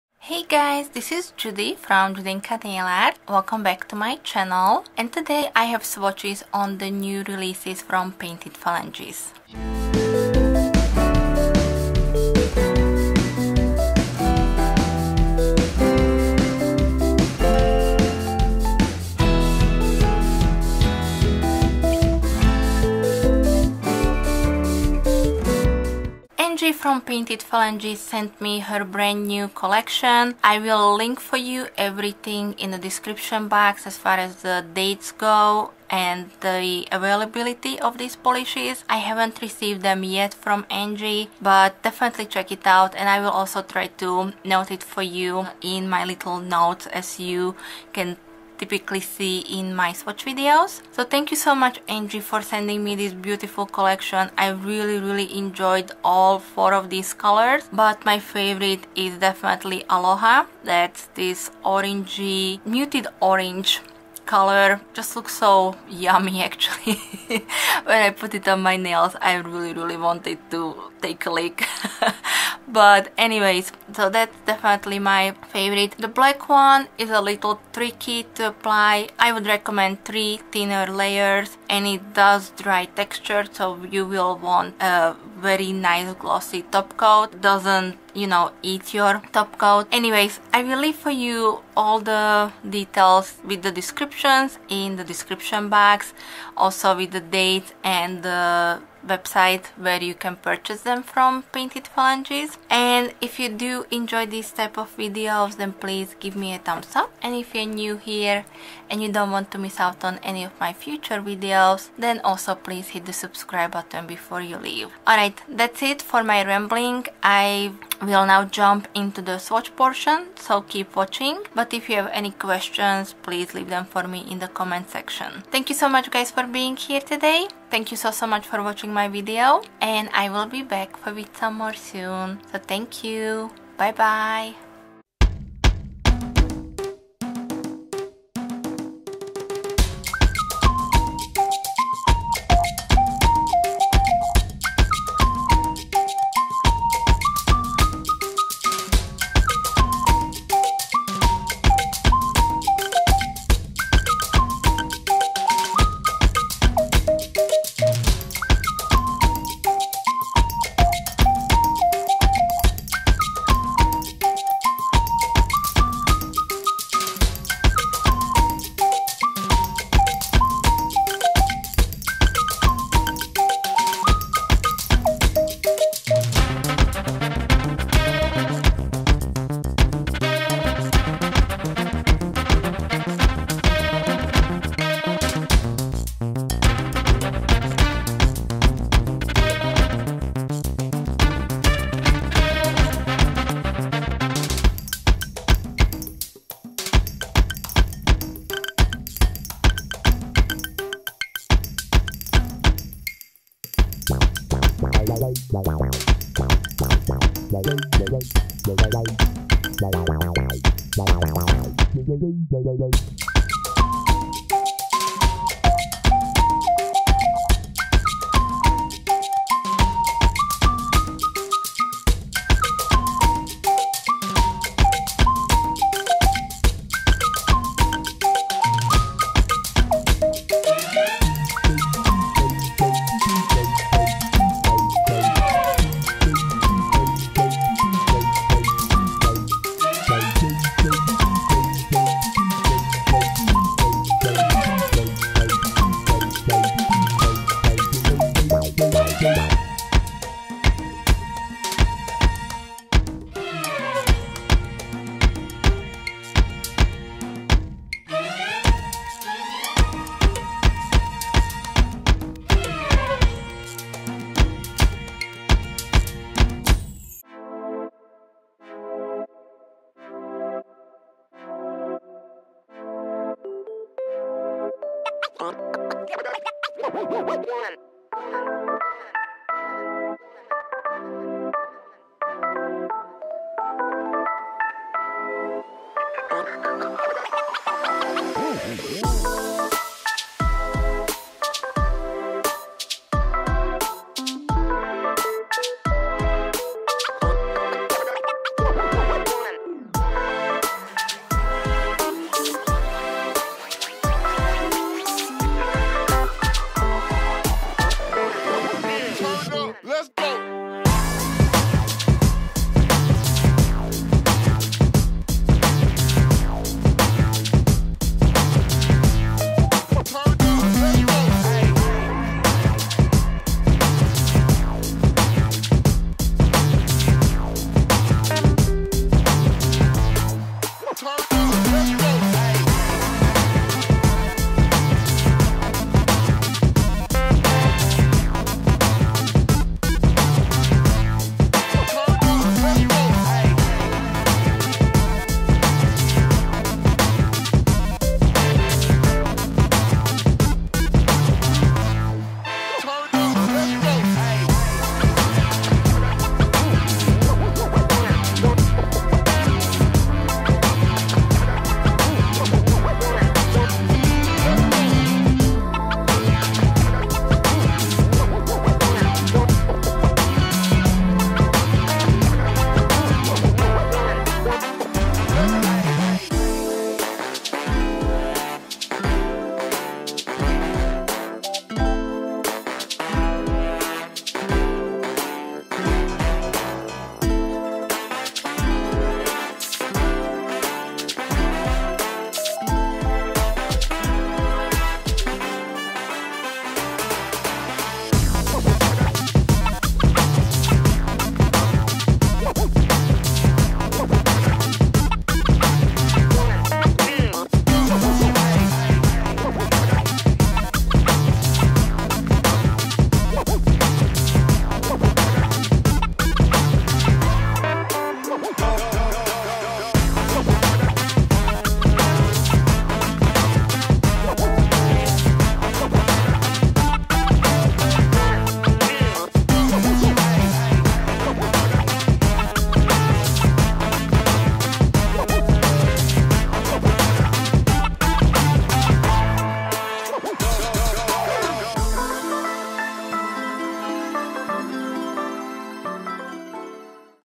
Painted Phalanges The Island Time Collection | Review & Swatches
OPEN MEEEE!
These polishes were sent to me by Painted Phalanges for review.

Hey guys, thank you for being here and watching my video. Below you can find all the details to my video.
In this video:
These are the Painted Phalanges August 2020 nail polish releases. I received the Island Time collection polishes for review from Painted Phalanges and the swatches are on my natural nails. I absolutely love them and would recommend all of them if you interested in purchasing them. They will be available starting August 19, 2020; 4/$40 or $11/each. If you are interested in purchasing them, please check out Angie's store:

*Rainbow Fish (Uhu) is a tropical teal blue that gets it’s rainbow fish scale look with 2 different kinds of shifty microflakies, white shreds, and pink, blue, purple, and platinum microflakies.
*Ali’i is a muted sunset purple loaded with gold/green/oranges/red/yellow shifty microflakies and blue microflakies.
*Aloha is a big sister polish to last summer's Aloha oe. It’s an amped up creamsicle orange loaded with blue/purple/pink/orange/yellow shifty microflakies, bronze microflakies, and a red/pink/orange shifty shimmer.
*Salty Beaches is a black jelly polish filled with black holo microglitter, black iridescent glitter, and some silver holo glitter speckled in.